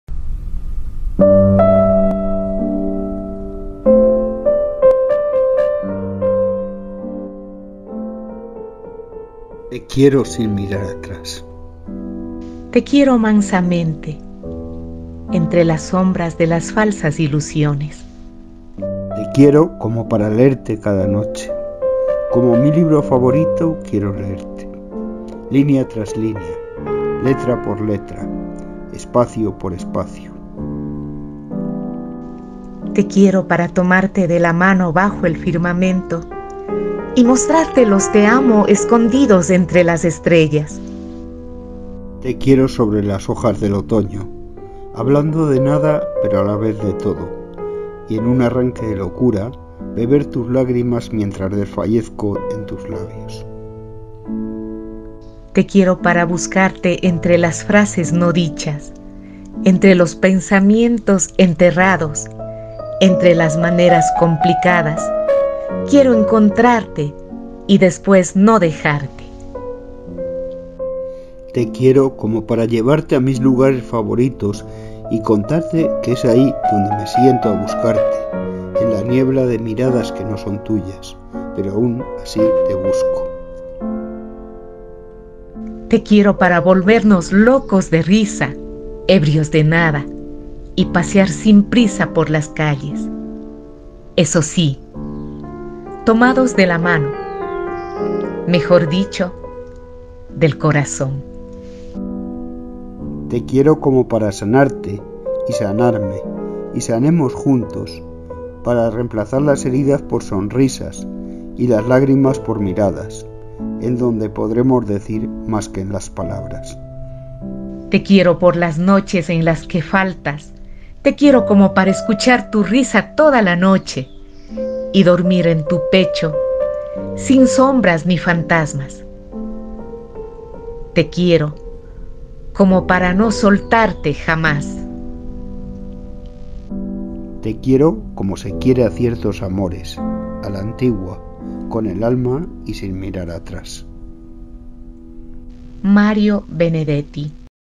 Te quiero sin mirar atrás. Mario Benedetti
Mario Benedetti un escritor, poeta, dramaturgo y periodista uruguayo, integrante de la Generación del 45.